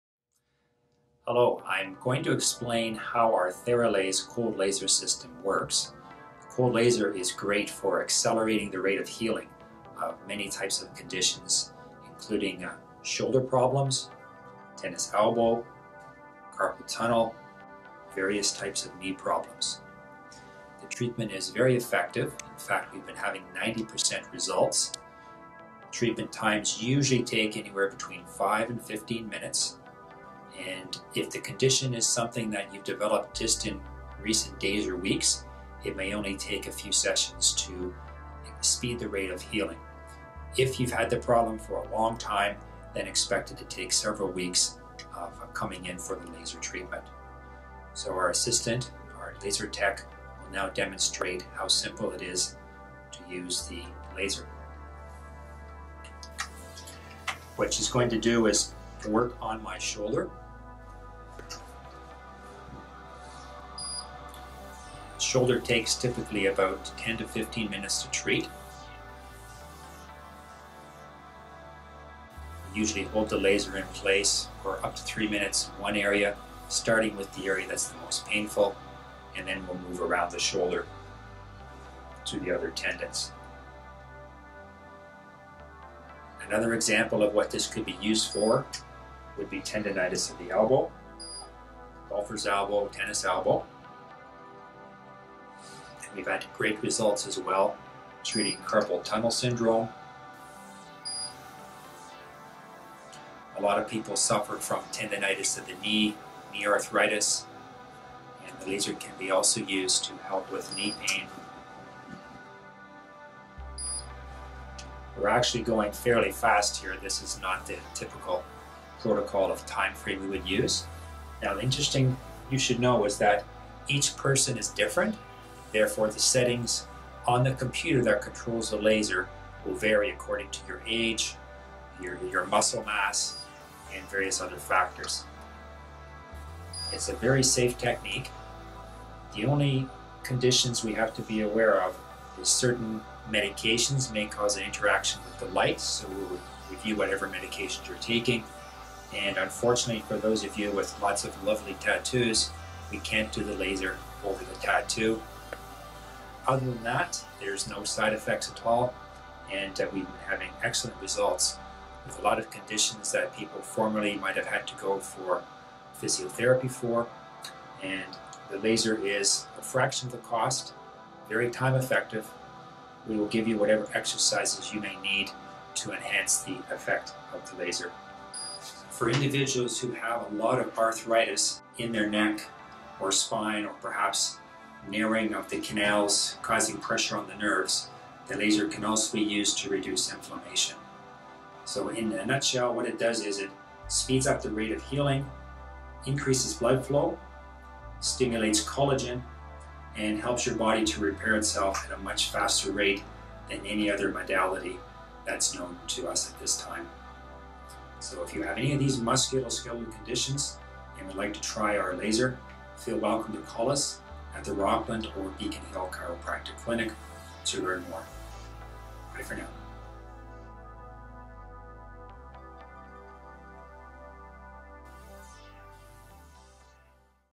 Theralase Cold Laser System in Chiropractic
Hello! I'm going to explain how our Theralase cold laser system in chiropractic works.  Cold laser is great for accelerating the rate of healing of many types of conditions including shoulder problems, tennis elbow, carpal tunnel, various types of knee problems.  The treatment is very effective, in fact we've been having 90% results. Treatment times usually take anywhere between five and fifteen minutes and if the condition is something that you've developed just in recent days or weeks it may only take a few sessions to speed the rate of healing.

If you've had the problem for a long time, then expect it to take several weeks of coming in for the cold laser treatment.  So our assistant, our laser tech, now will demonstrate how simple it is to use the cold laser.  What she is going to do is work on my shoulder.  A shoulder takes typically about 10 to 15 minutes to treat.  Usually you hold the laser in place for up to three minutes one area, starting with the area that's the most painful, then we'll move around the shoulder to the other tendons.

Another example of what this could be used for would be tendonitis at the elbow, golfer's elbow, tennis elbow... We've had great results as well treating carpal tunnel syndrome.  A lot of people suffered from tendinitis of the knee, knee arthritis and the laser can be also used to help with knee pain.  We're actually going fairly fast here, this is not the typical protocol of timeframe we would use.

Now interesting you should know is that each person is different therefore the settings on the computer that controls the laser will vary according to your age, your muscle mass and various other factors.  It's a very safe technique.  The only conditions we have to be aware of is certain medications may cause an interaction with the lights, so we would review whatever medications you're taking and unfortunately for those of you with lots of lovely tattoos, we can't do the cold laser over the tattoo.  Other than that, there's no side effects at all.

We are having excellent results with a lot of conditions that people formerly might have had to go for physiotherapy for and the laser is a fraction of the cost, very time effective, we will give
you whatever exercises you may need to enhance the effect of the laser.

For individuals who have a lot of arthritis in their neck or spine or perhaps narrowing of the canals causing pressure on the nerves, the cold laser can also be used to reduce inflammation.  So in a nutshell what it does is it speeds up the rate of healing, increases blood flow, stimulates collagen and helps your body to repair itself at a much faster rate than any other modality that's known to us at this time.

So if you have any of these musculoskeletal conditions and would like to try our laser, feel welcome to call us at the Rockland Chiropractic Clinic or Beacon Hill Chiropractic Clinic to learn more about the Theralase Cold Laser System.